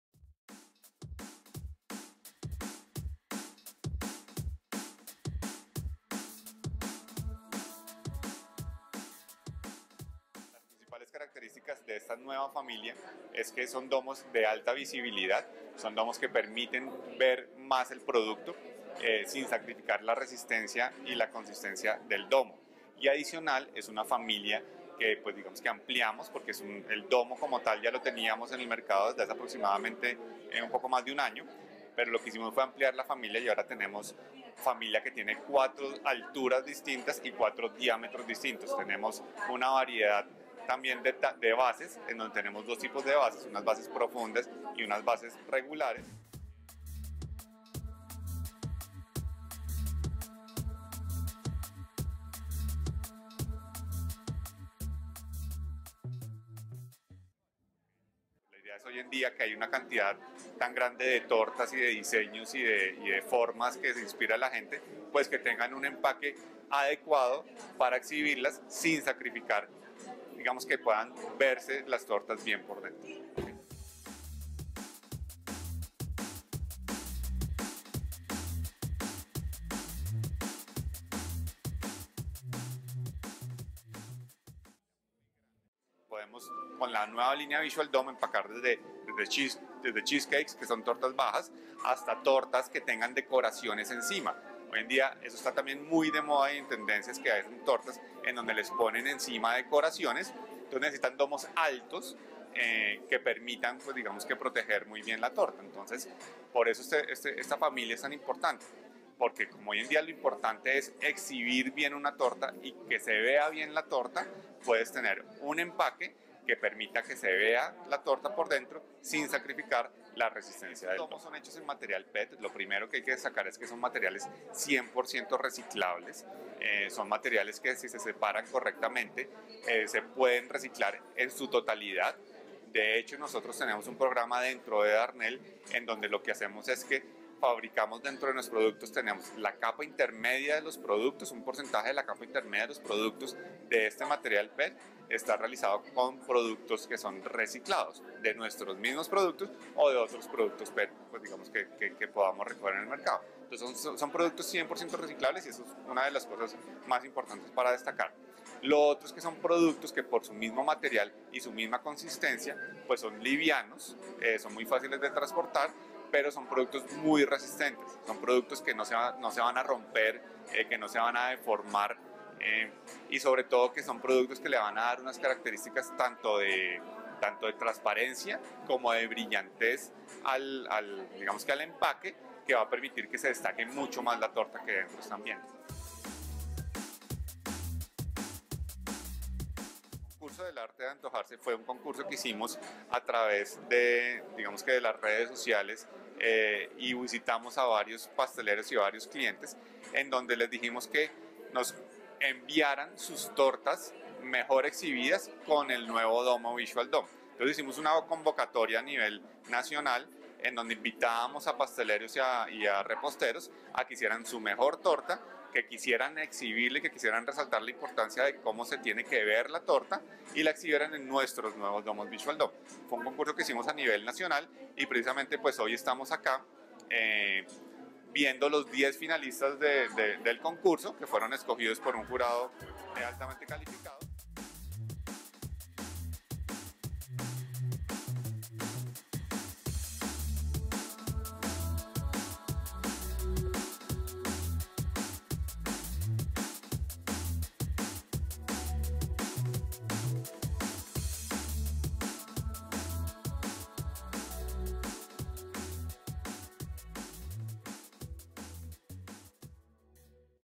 El Arte de Antojarse, concurso de Darnel
En días pasados, Darnel presentó la ampliación de su línea de domos Visual Dome y premió los mejores pasteles exhibidos en estas nuevas soluciones. Vea mas información en: